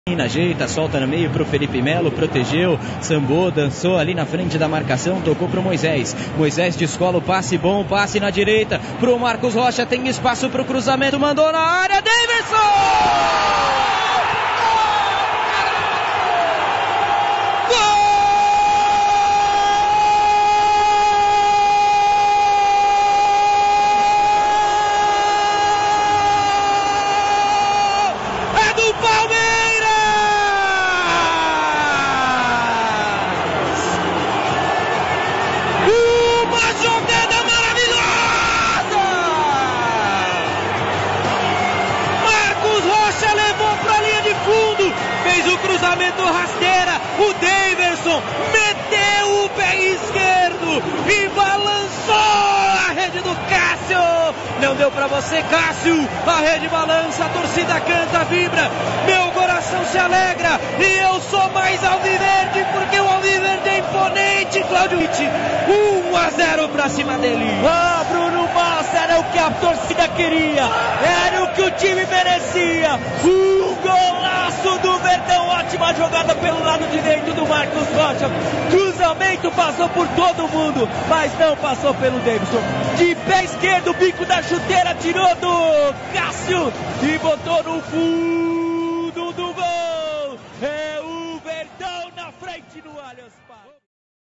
Gol de Deyverson, Palmeiras 1 x 0 Corinthians
Não adiantou o juiz ser o péssimo dos péssimos.
Não adiantou o time pequeno (o dó da formiguinha diria a outra) vir todo fechado.
Chupa freguesia fiel.

Confira na voz de Bruno Massa a narração do gol da vitótia .

APOIE: Apoie a rádio acesse o link e deixe sua colaboração

Acesse e veja como apoiar a rádio com alguns cliques.